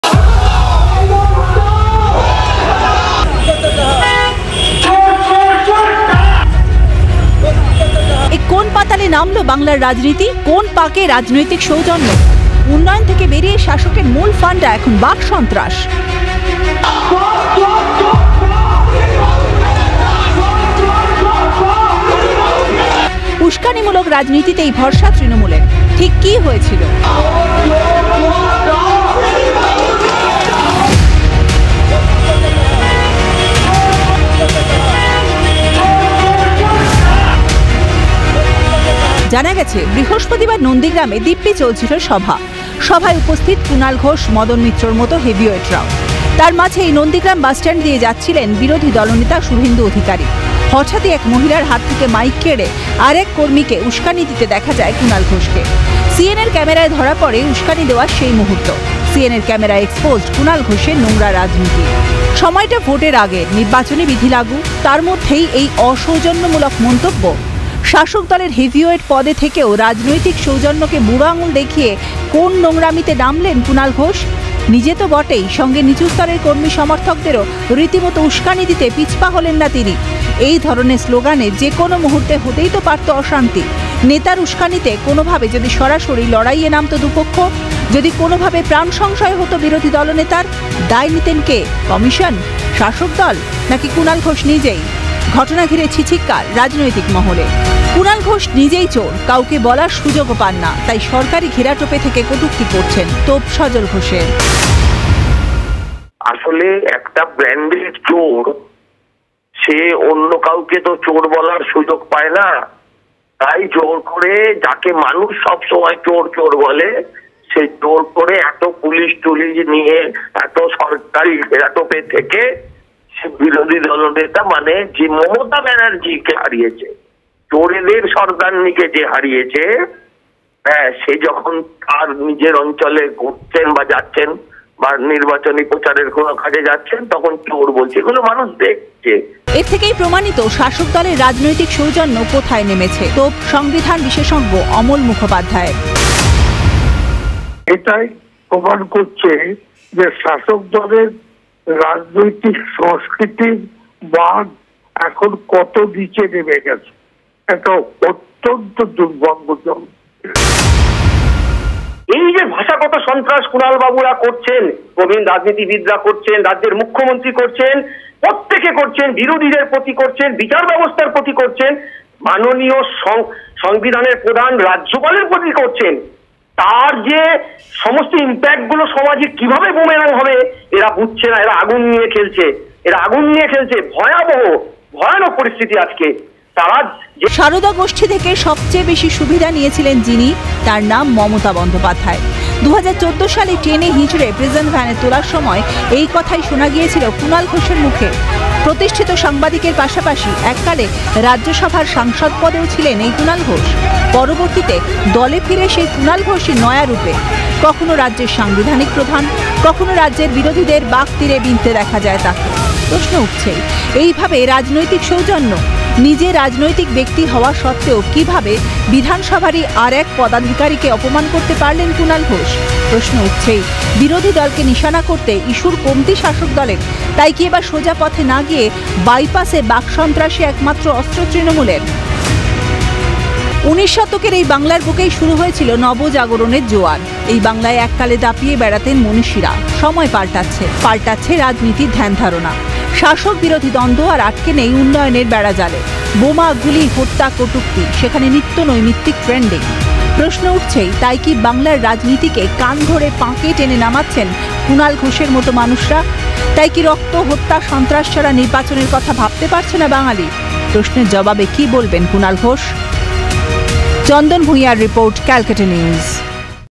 কুণালের উস্কানি EXPOSED ? | ক্যালকাটা নিউজ
শুভেন্দুকে 'চোর' স্লোগান দিতে উস্কানি। সিএনের ক্যামেরায় কুণালের উস্কানি EXPOSED ? এ কোন পাতালে নামল বাংলার রাজনীতি? কোন পাঁকে রাজনৈতিক সৌজন্যতা? উন্নয়ন থেকে বেরিয়ে শাসকের মূল ফান্ডা এখন বাক সন্ত্রাস। ছিঃ ছিঃ কার গোটা বাংলায়।